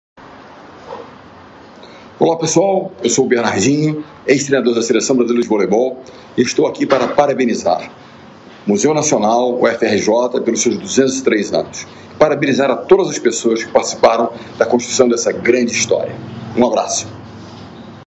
Técnico Bernardinho fala sobre os 203 anos do Museu Nacional
Ex-técnico da seleção brasileira de vôlei parabeniza UFRJ e Museu Nacional pelo aniversário e importância histórica.